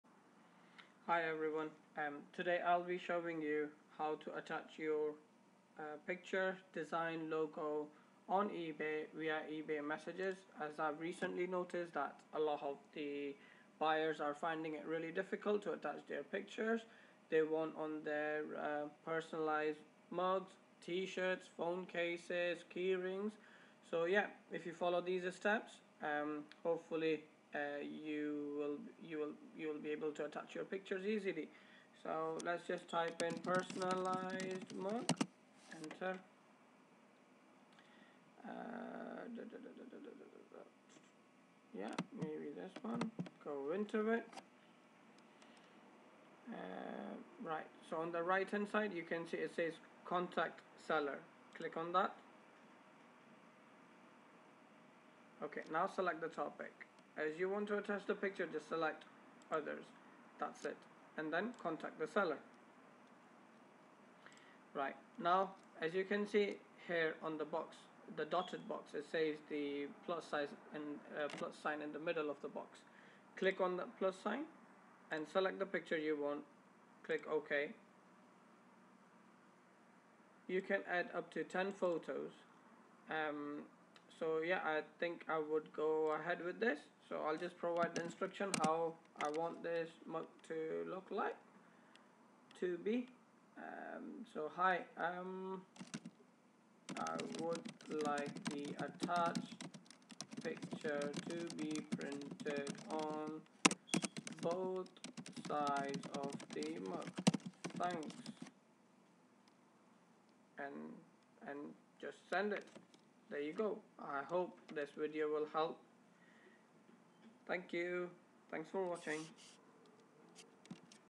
How to upload / attached your Pictures / Logo / Artwork / Design on eBay via eBay messages
In this video we have tried to explain how an eBay buyer can attached their picture or any other artwork which they want to be printed on customised products.